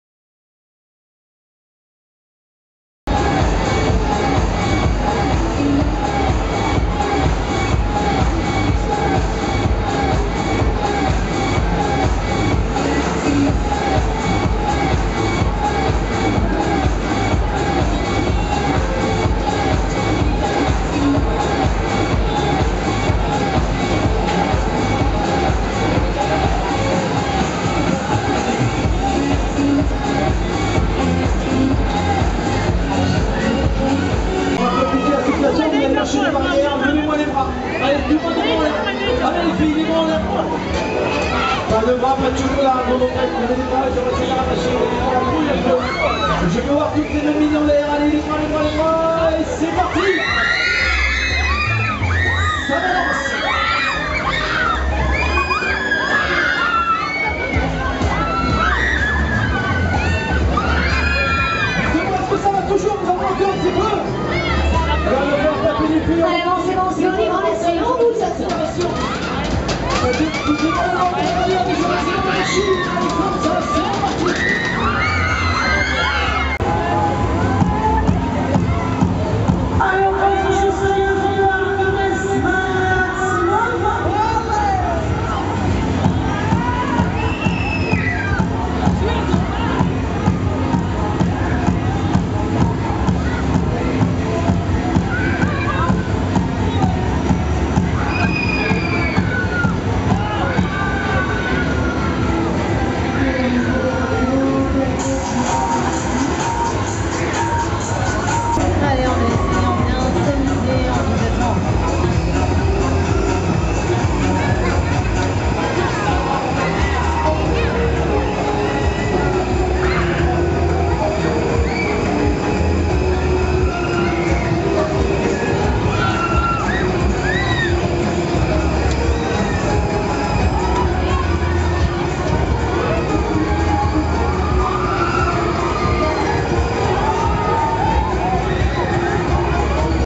Fête foraine de Bayonne (64)
La foire attractive de Bayonne le dimanche 12 avril 2015. Métiers présentés : L'experience, Crazy Dance, La Pieuvre et le surf Free Style.